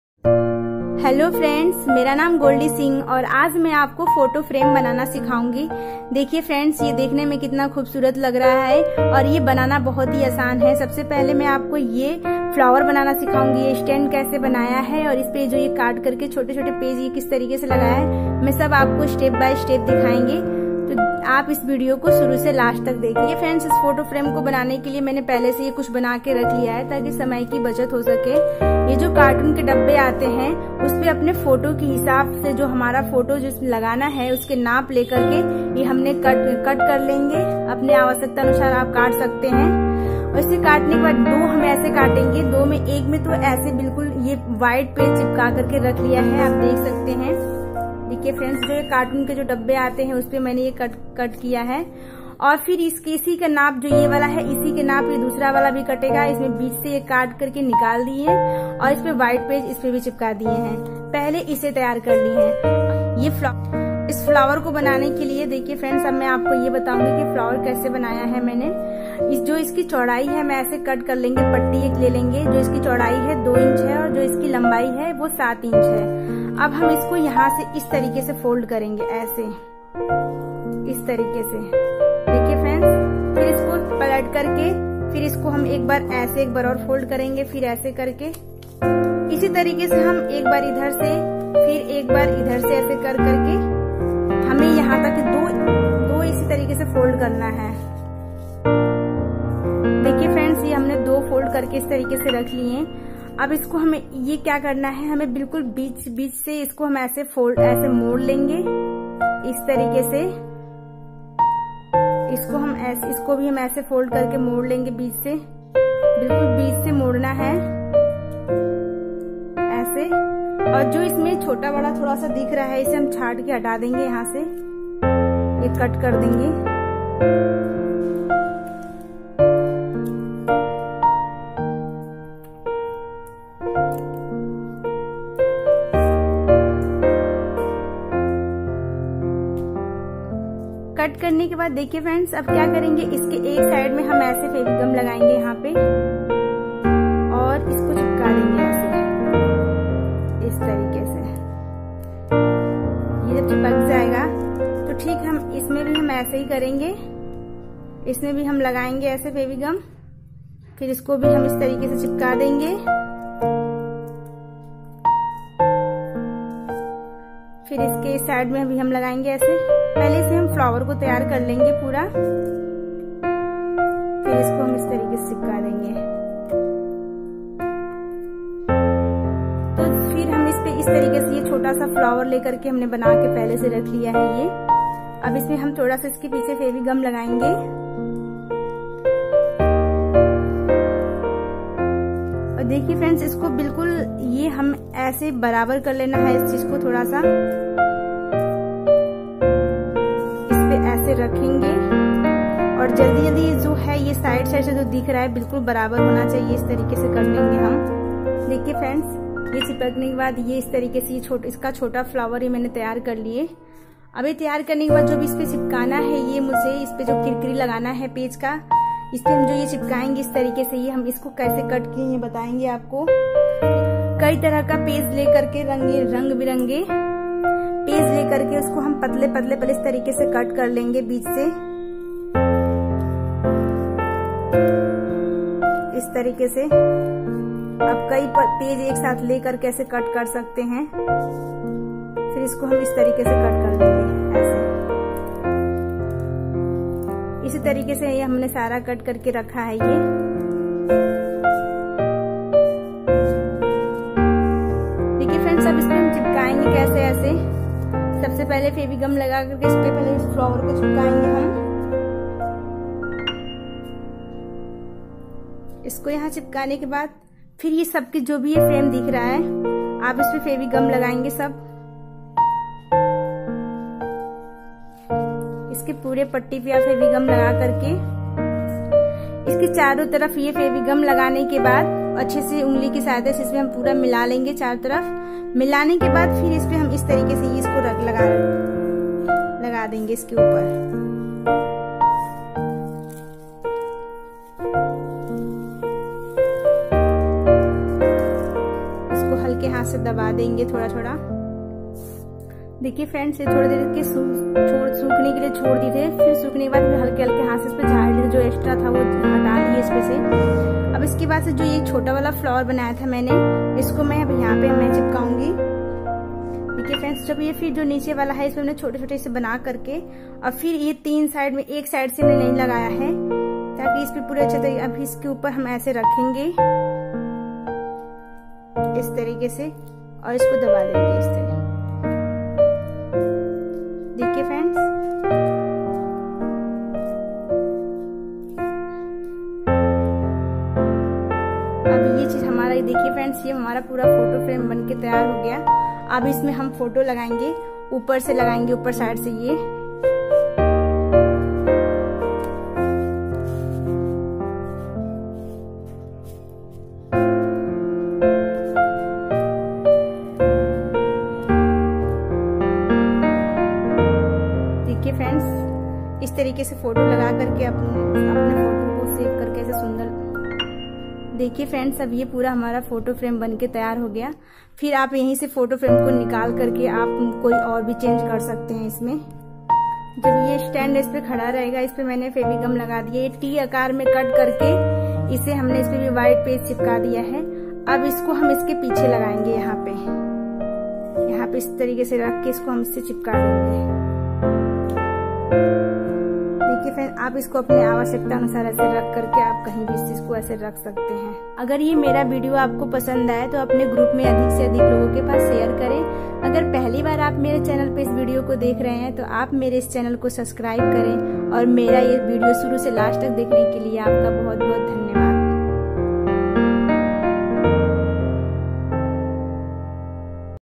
Paper Photo Frame | पेपर फोटो फ्रेम | Gift Ideas
Friends,
Lets learn  to make a beautiful photo frame made of paper. It is a perfect  gift to give to your dear ones.  Hope you will make and gift them to your dear ones.